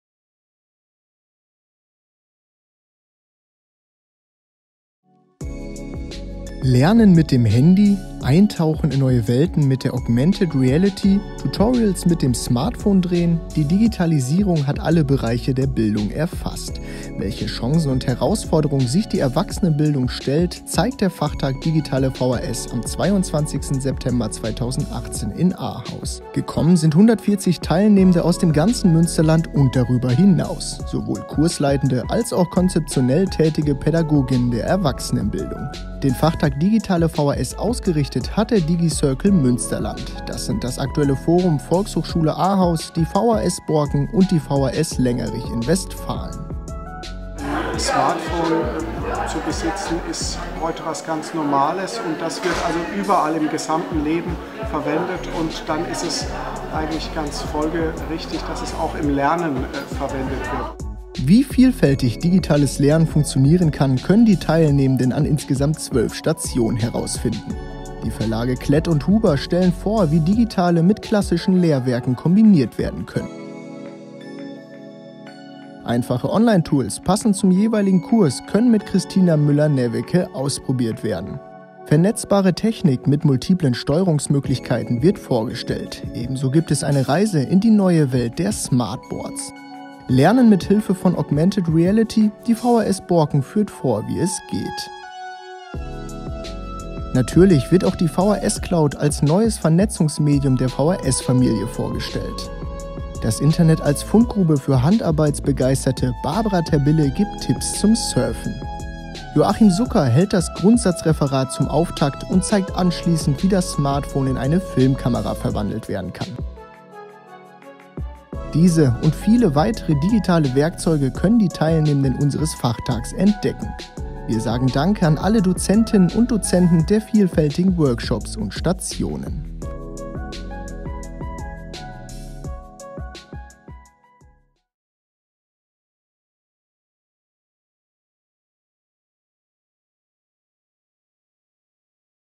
Fachtag "Digitale vhs" 22.09.2018
Am 22. September 2018 hat der Digi-Circle Münsterland (aktuelles forum Volkshochschule Ahaus, vhs Borken, vhs Lengerich) einen Fachtag "Digitale vhs" für das Münsterland ausgerichtet. Dieser Film fasst unser "Leuchttorumprojekt" gut zusammen. Viel Spaß dabei!

Film: Patrick Gillitzer